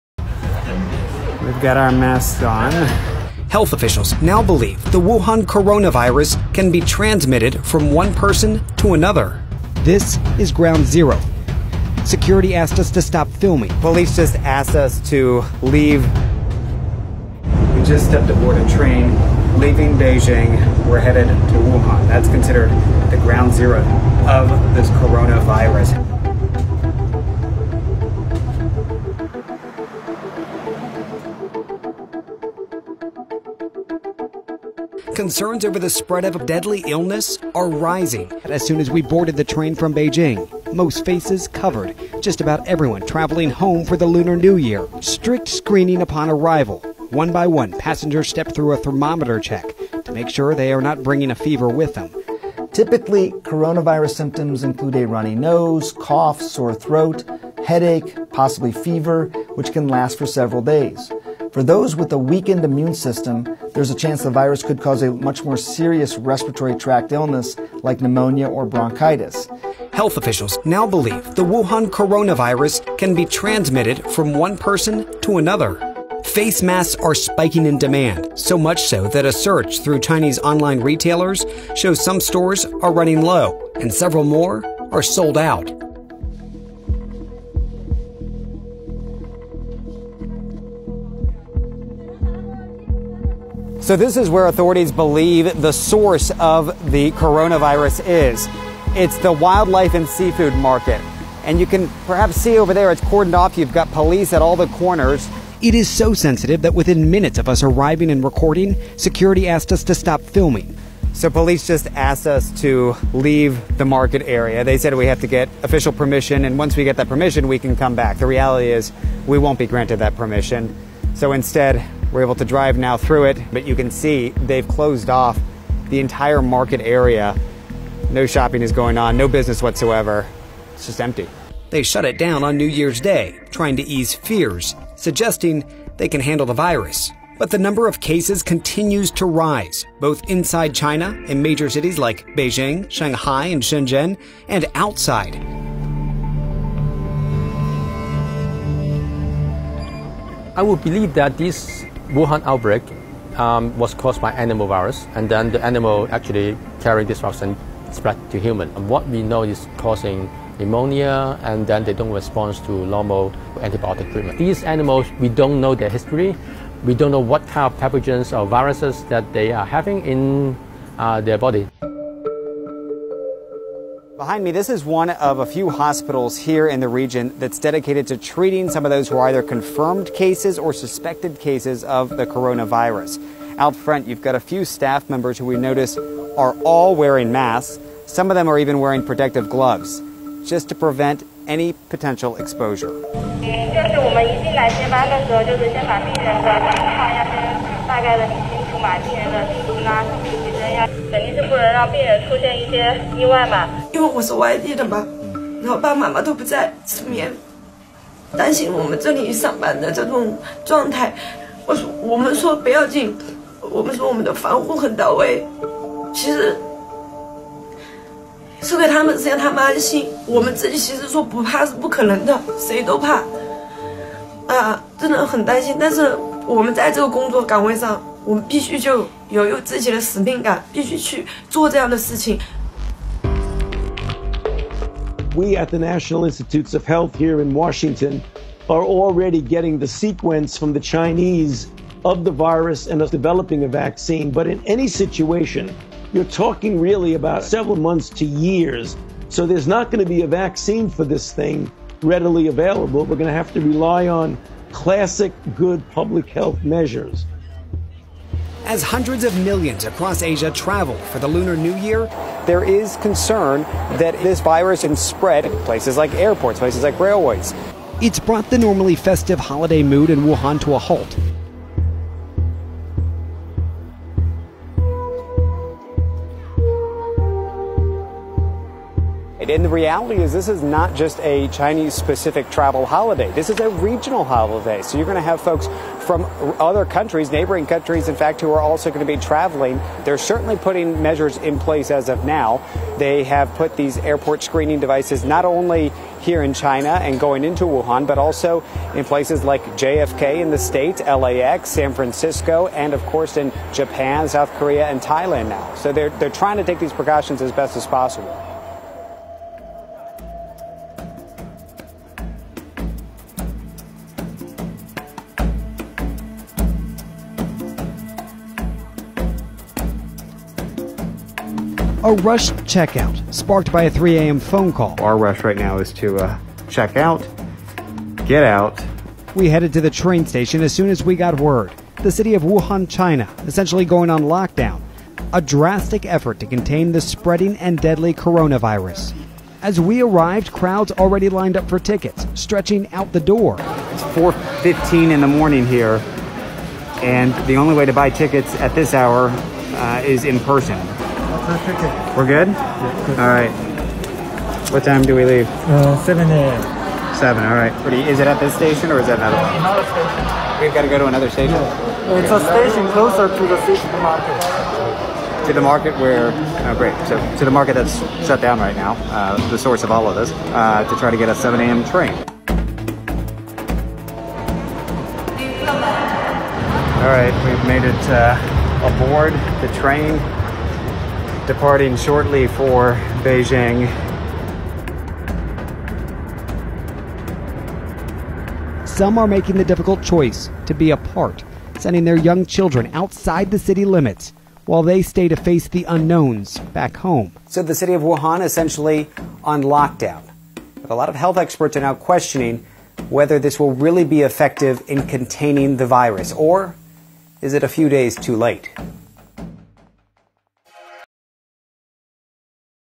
Fear and anxiety in epicenter of Wuhan coronavirus outbreak
China is restricting travel of 30 million people as the coronavirus death toll rises to at least 26, with more than 900 confirmed cases worldwide. CNN’s David Culver traveled to Wuhan, the epicenter of the outbreak, and scrambled to flee the city before authorities placed the city of 11 million on a partial lockdown.